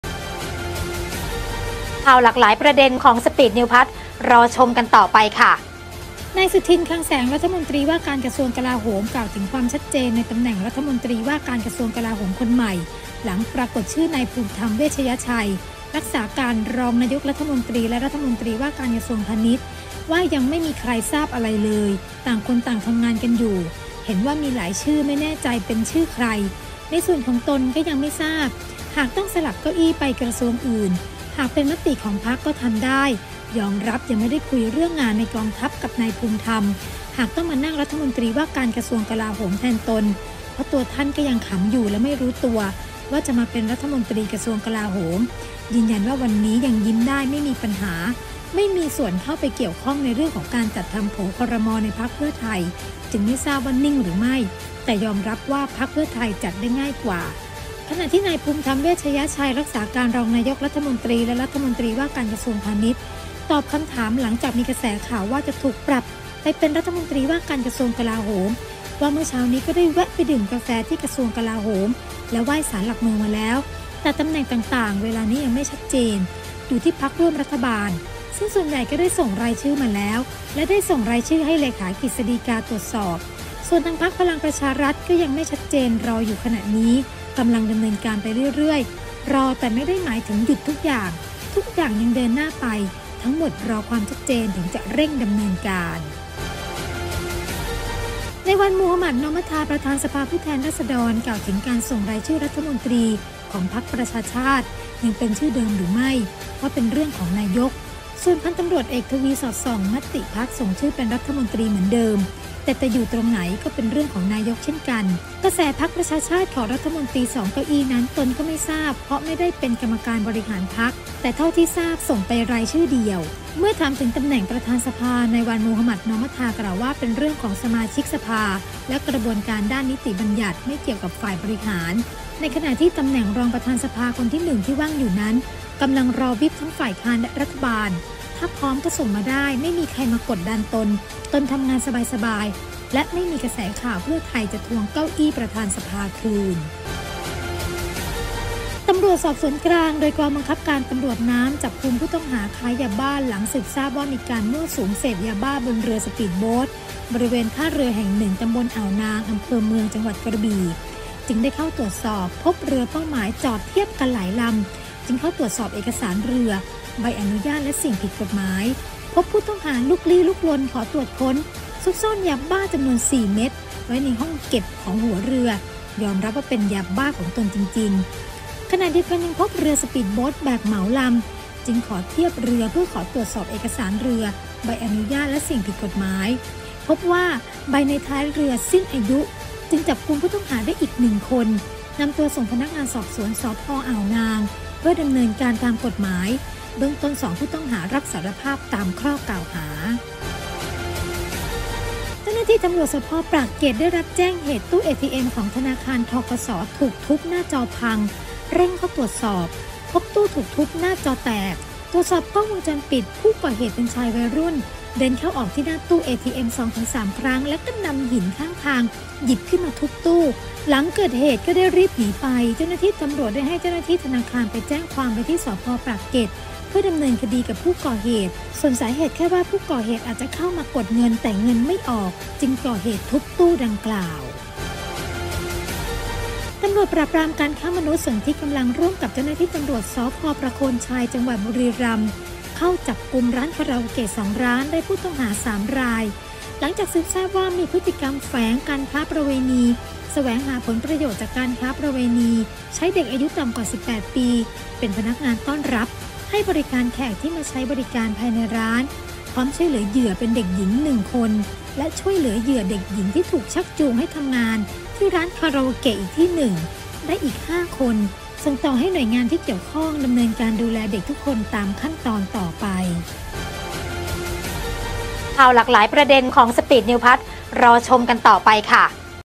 TNA SPEED NEWS plus+ |22 สิงหาคม67| : 14.00น.
1 “สุทิน” ยังยิ้มได้หลังชื่อ “ภูมิธรรม”เสียบเก้าอี้ กลาโหม

2 “วันนอร์” ยืนยันไม่ใครกดดันการทำงาน (กทม)

3 ตำรวจน้ำ รวบเด็กเรือ ค้ายาเสพติด (กระบี่)

4 เหตุคนร้ายทุบตู้ เอทีเอ็ม ธนาคาร ธกส. หน้าจอเสียหาย (นนทบุรี)

5 ตร.ปคม.ทลายคาราโอเกะ ค้ามนุษย์ (บุรีรัมย์)